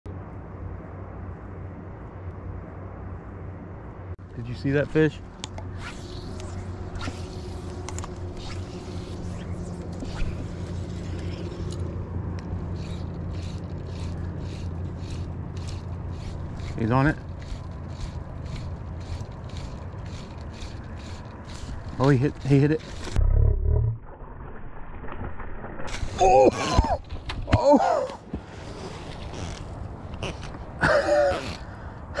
Tarpon SMASHING Fly
Florida Keys Juvenile Tarpon Action @SpaceCoastFlies